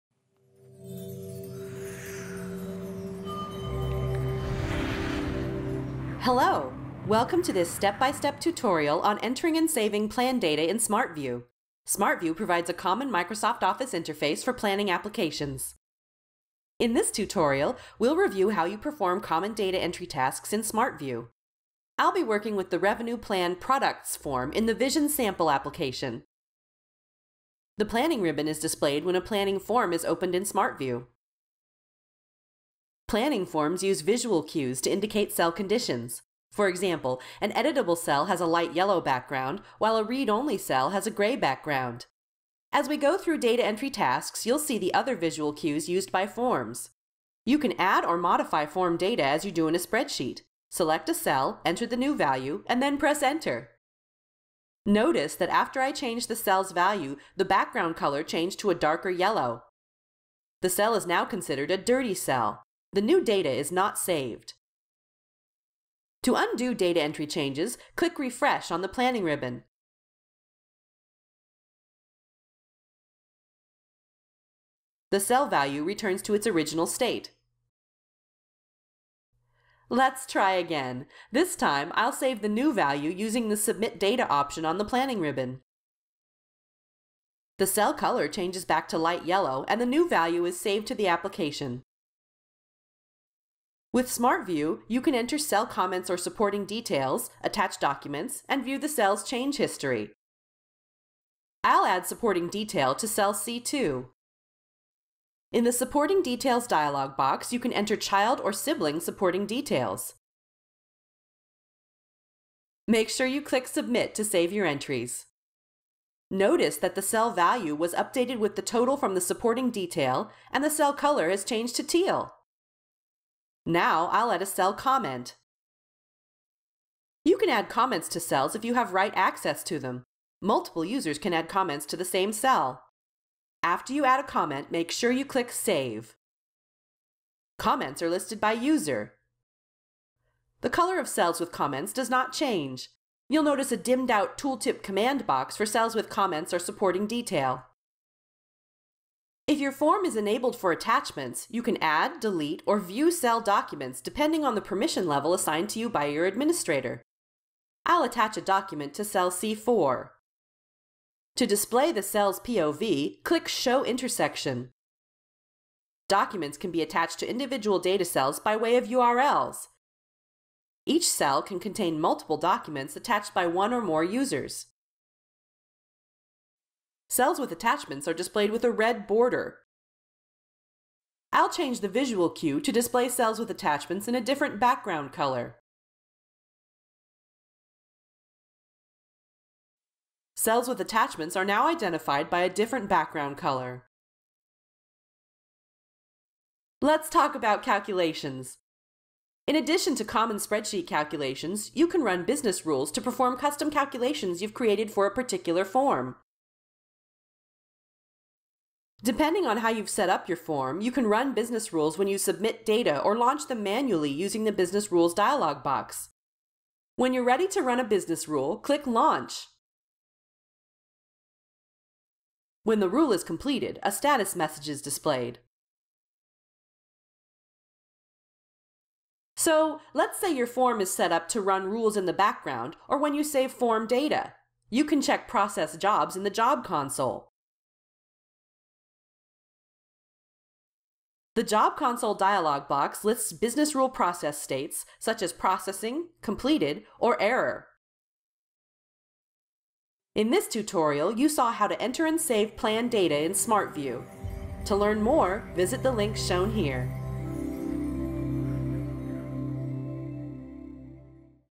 Entering and Saving Plan Data in Smart View with Oracle Planning and Budgeting Cloud
This tutorial demonstrates how to enter and save application plan data in Oracle Planning and Budgeting Cloud using Smart View. You learn how to perform common data entry tasks in Smart View. You also learn how to discern visual cues that indicate cell conditions. You see how to add or modify form data, enter supporting details and comments, view the job console, and launch calculation rules.

Planning and Budgeting Cloud is now Planning, a business process in Oracle EPM Cloud